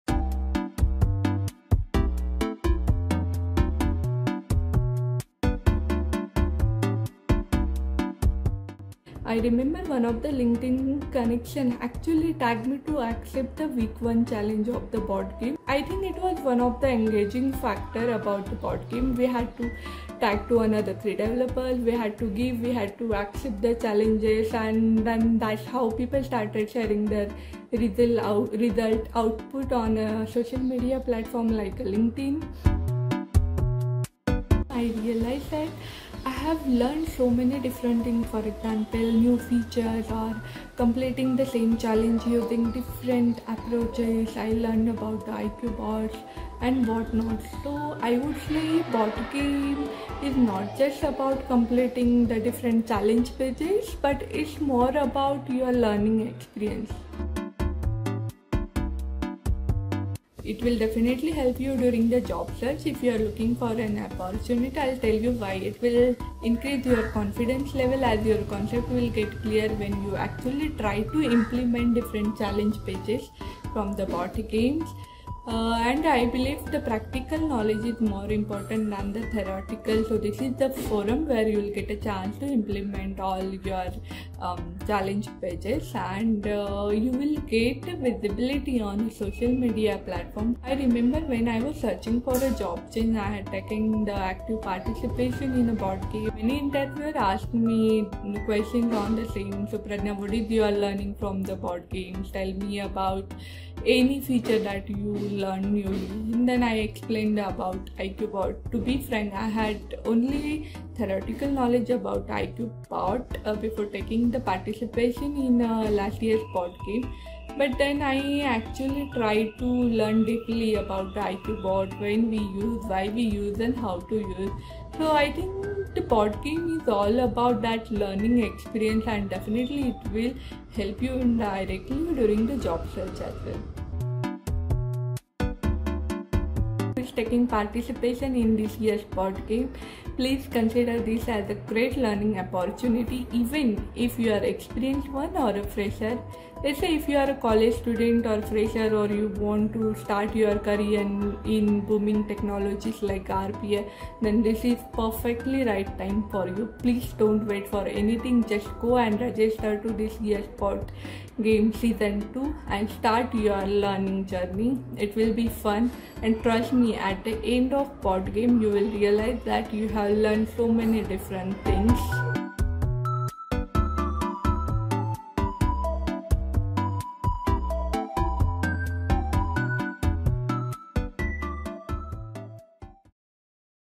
What Bot Games Means to Me | Pradnya Mind
Bot Games is back with Bot Games Season 2! Why does Automation Anywhere even do Bot Games in the first place though? For our Developer Community! Bot Games is a great opportunity for both new and experienced users to learn and practice using Automation 360 more effectively. In this video, we hear from one of those community members – Pradnya Mind – on her experience with Bot Games and what it’s meant for her both personally and professionally.

Be Sure to Register for This Year’s Games to be Eligible to Win Swag:

00:00 – Start of video
00:07 – How Did you First Learn About Bot Games?
00:33 – How Has Bot Games Helped You As a Developer
00:59 – How Has Bot Games Helped You Professionally?
02:15 – What Advice Do You Have For Others Interested In Bot Games?